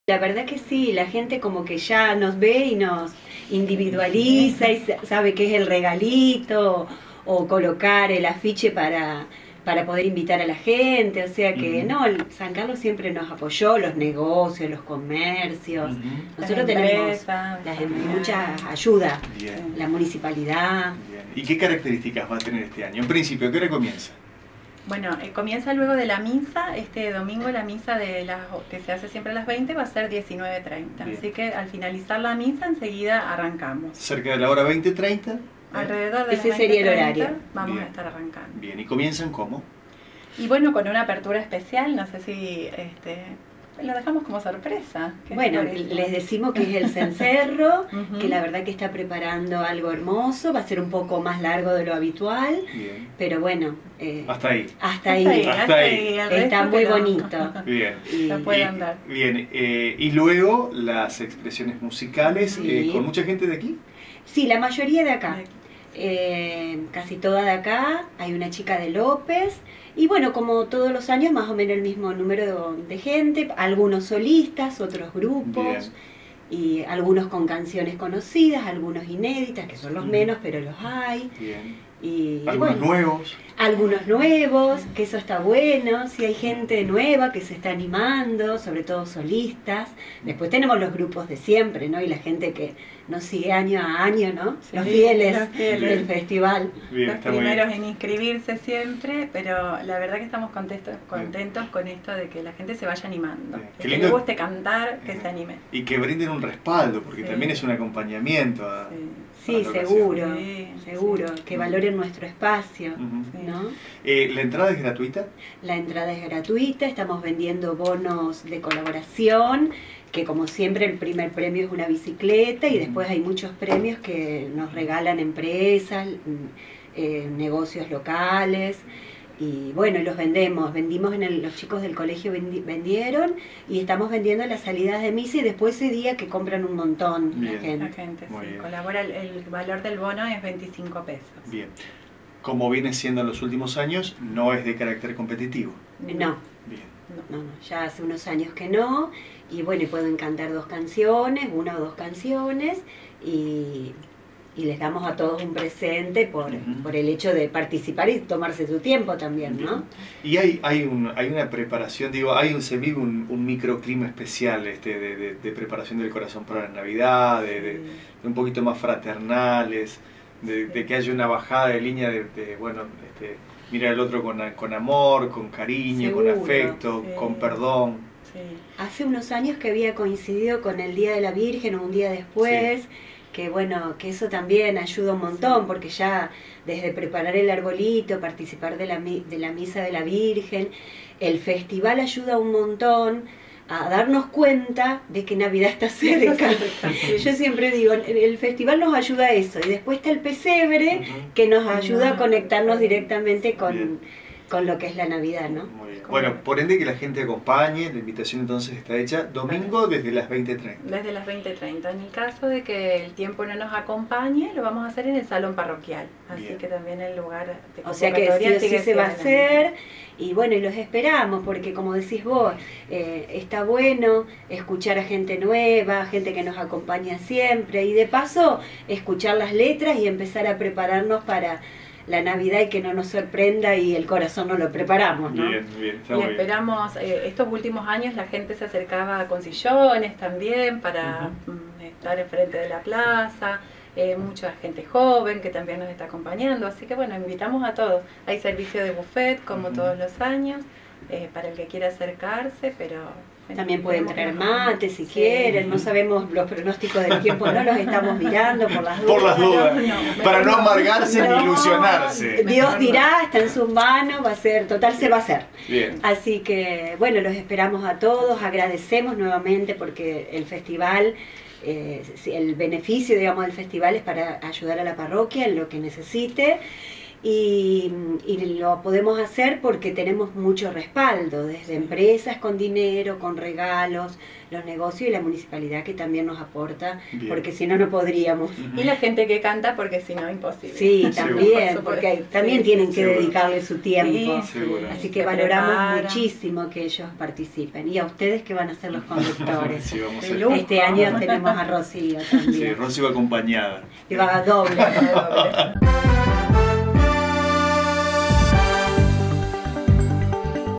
Erica Eberhardt – Marisa Frigeri
El próximo domingo se realizará el Festival de la Canción Navideña